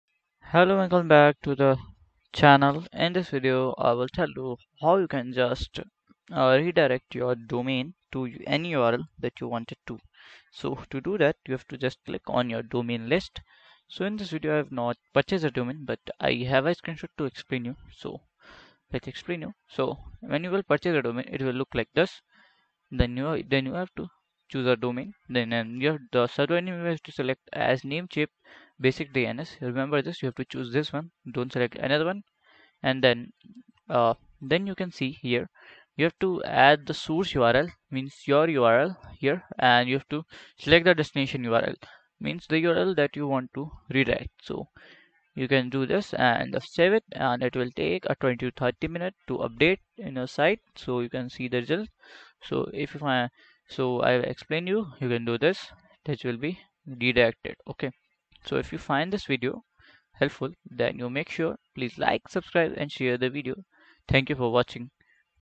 ✅ How To Redirect Namecheap Domain To URL (Full Guide)
Elevate your online strategy with our step-by-step guide on redirecting your Namecheap domain to a specific URL! 🌐🔄 Whether you're rebranding, updating links, or streamlining your online presence, this comprehensive tutorial ensures a seamless redirection process.

🔍 Key Learning Points:
  • Detailed walkthrough on redirecting a Namecheap domain to a specific URL
  • Pro tips for efficient and strategic redirection
  • Essential information to ensure a hassle-free domain-to-URL redirection